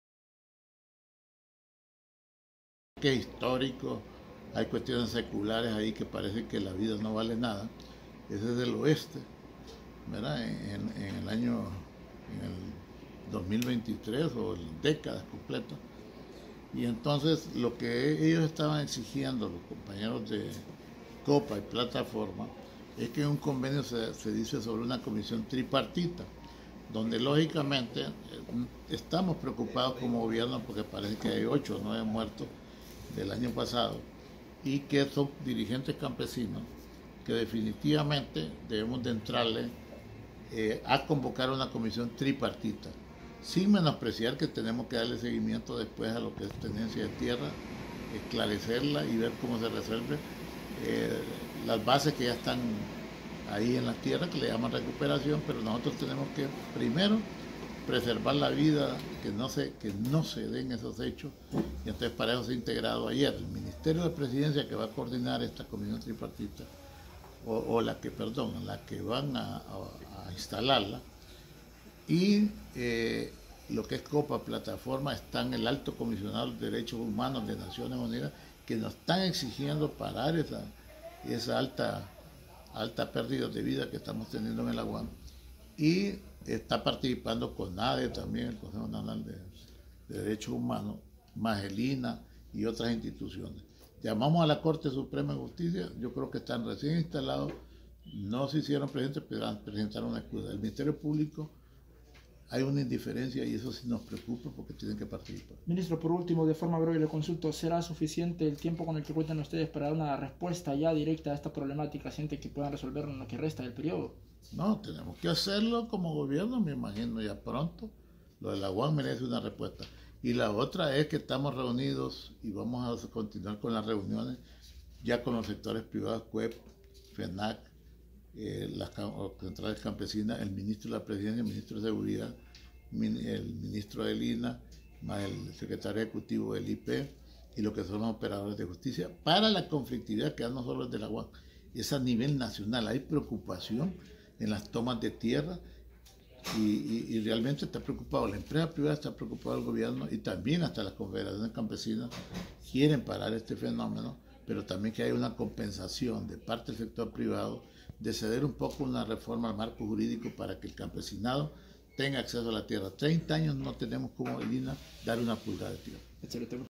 INA INSTALA COMISIÓN TRIPARTITA PARA RESOLVER CONFLICTO AGRARIO
Instalan y presiden Comisión Tripartita, con el objetivo  de buscar soluciones a la conflictividad agraria que ha existido durante años en el Bajo Aguán.

Misma que ha realizado un análisis sobre las causas y concecuencias de la conflictividad socioambiental y agraria, que ha dejado como resultado cientos de campesinos asesinados y familias empobrecidas, junto a la ONU se logró relanzar la Comisión Tripartita que investigará y dará seguimiento a estos casos.